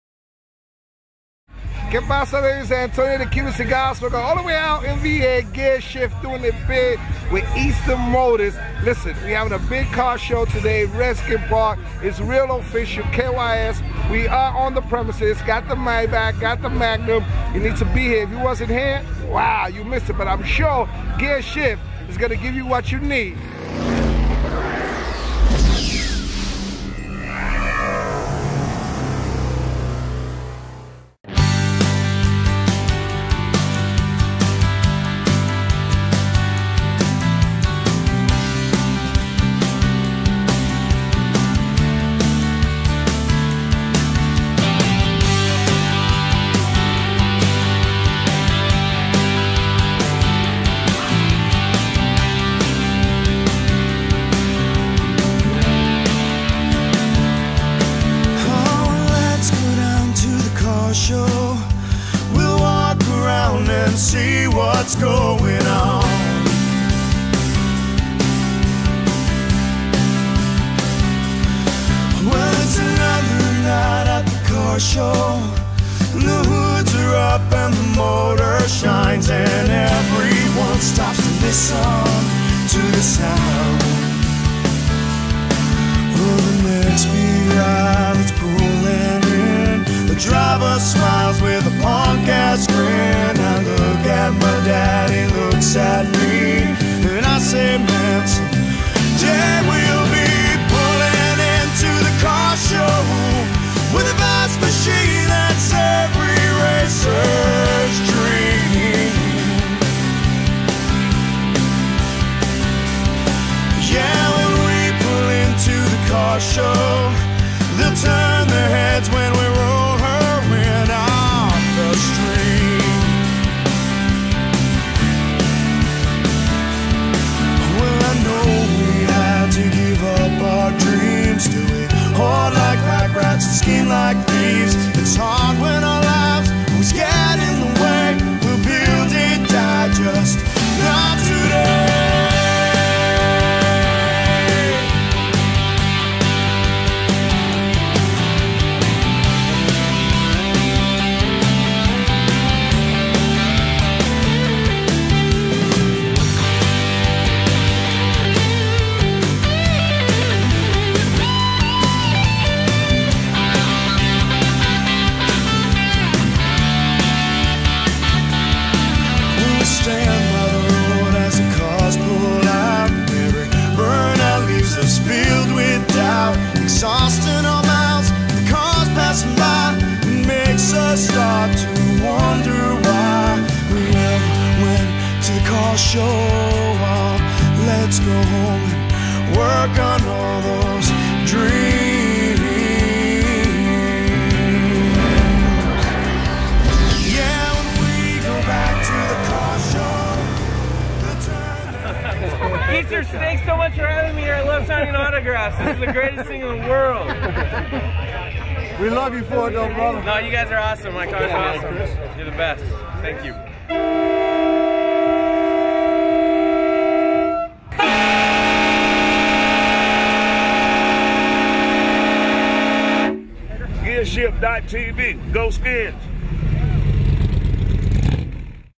Albert Haynesworth Smiles at the Redskins Rides 2010 Car Show by Easterns.com
- Albert Haynesworth, Chris Cooley, Chris Samuels, Gary Clark & Chief Zee were celebrating another season of football fun with the annual Redskins Rides 2010 car show at Redskins Park.  Gearshift captured stunning 1080p video of the event.